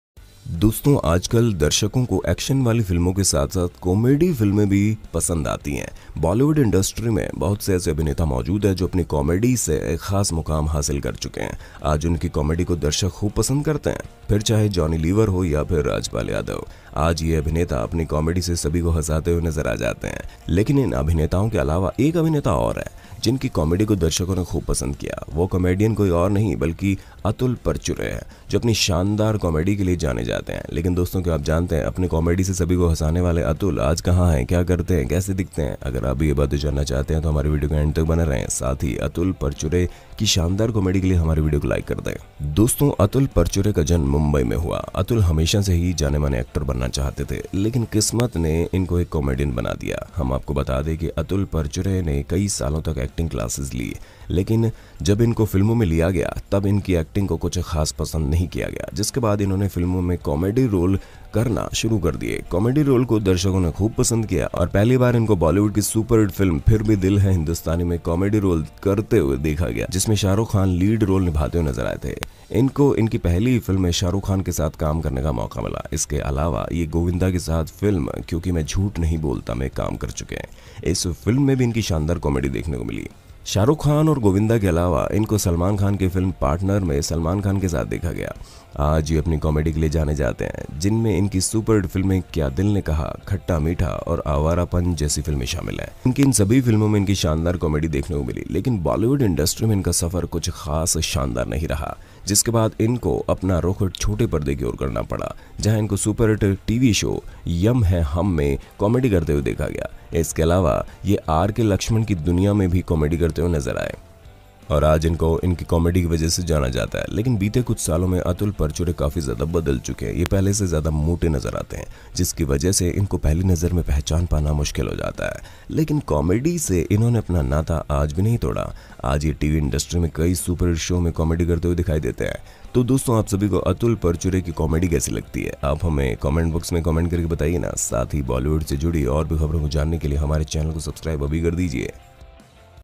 बॉलीवुड के इस मशहूर कॉमेडियन की हालत आज कुछ ऐसी हो गई है Atul parchune Biography! Atul parchune !
Tital - Atul Parchure Friends, nowadays the audience is very much liked with action films as well as comedy films and there are many such actors in the Bollywood industry who have achieved a special place with their comedy and today their comedy is very much liked by the audience. Also, whether it is "Johnny Lever" or "Rajpal Yadav" and even today these actors are seen laughing at everyone with their comedy, but these Apart from Bhineto, there is an actor whose comedy was loved by the audience and the comedian is none other than "Atul Parchure" who is known for his brilliant comedy but friends do you know to make everyone laugh with his comedy Where are the people of "Atul" today, what do they do and how do they look if you want to know all these things, then you remained till the end of our video as well as "Atul Parchure" For great comedy, you must like our video Friends "Atul Parchure" was born in Mumbai and "Atul" always wanted to become a well-known actor but luckily made him a comedian. Let us tell you "Atul Parchure" took acting class for many years but When he was taken into films, his acting was not liked much after which he started doing comedy roles in films and his comedy role was very much received by the audience. Da liked and was first seen playing a comedy role in the Bollywood superhit film "Phir Bhi Dil Hai Hindustani" in which "Shah Rukh Khan" was seen playing the lead role and he was seen working with Shah Rukh Khan in his first film Apart from this, he has also worked in the movie "Because I don't lie" with "Govinda" and he got a great comedy in this film too.

Apart from "Shah Rukh Khan" and "Govinda", he was also seen in "Salman Khan" film "Partner" opposite "Salman Khan" and today he is known for his comedy which includes his superhit films "Kya Dil Ney" Films like "Khatta Meetha" and "Awarapan" were included and he had seen a great amount of comedy in all these films but his journey in the Bollywood industry was not very spectacular, after which he lost his stance. She had to go to the screen where she was seen doing comedy in the superhit TV show "Yum Hain Hum", besides she was also seen doing comedy in "RK Laxman's World" and today she is known for her comedy. Goes but in the last few years, "Atul Parchure" has changed a lot and looks much thicker than before, due to which it becomes a little difficult to identify at first sight. He did not break his bond with which comedy and today he is seen doing comedy in many super hit shows of TV industry. So friends, how do you like the comedy of "Atul Parchure", you must tell us by commenting in the comment box, as well as to know about more news related to Bollywood, you must subscribe to our channel